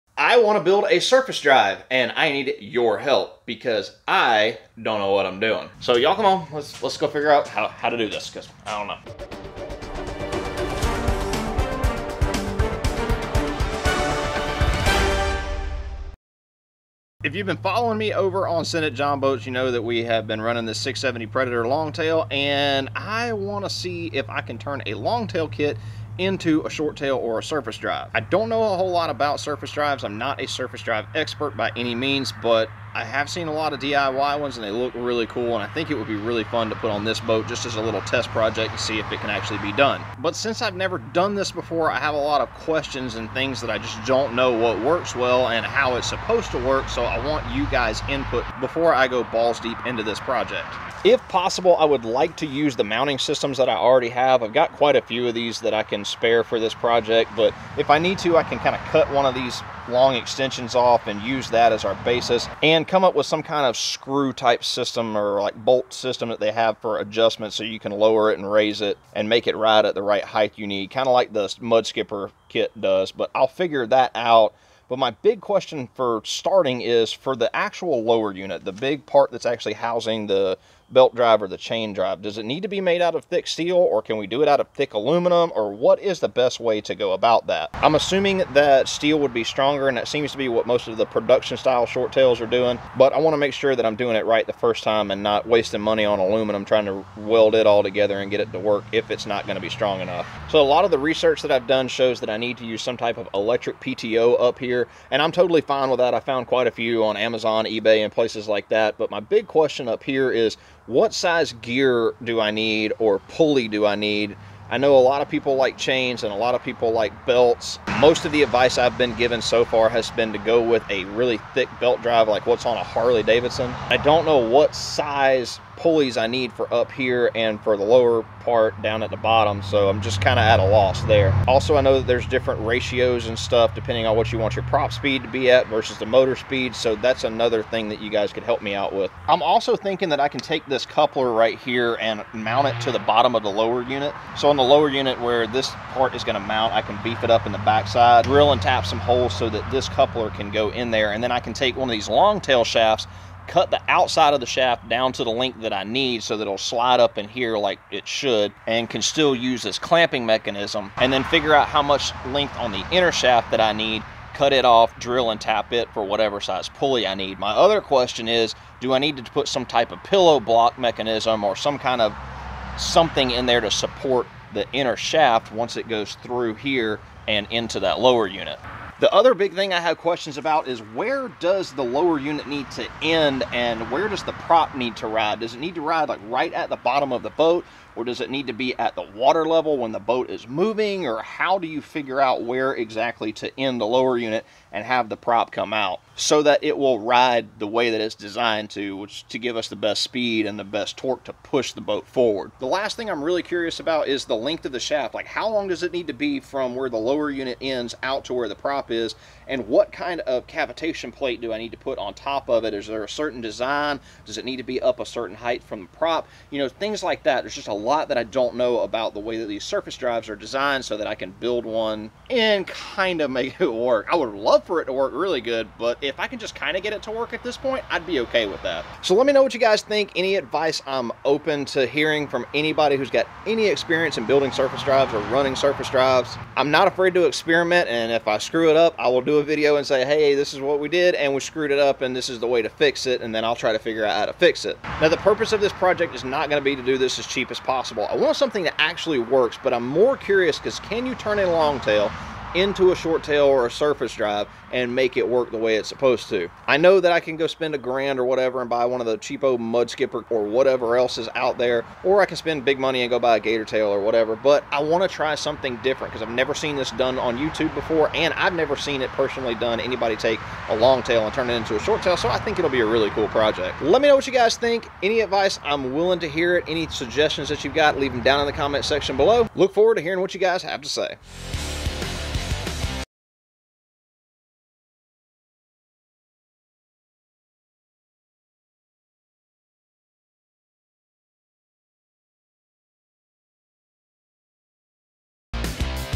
Can we build a DIY Short Tail Surface Drive Mud Motor?
In this video Chris from Send It Jon Boats is asking for advice and help with a new project... building a do-it-yourself surface drive or short tail mud motor. Help the guy out and give him some of your advice on building a good running mud motor.

Get 5$ off your OHW order use code: ATFHYDRO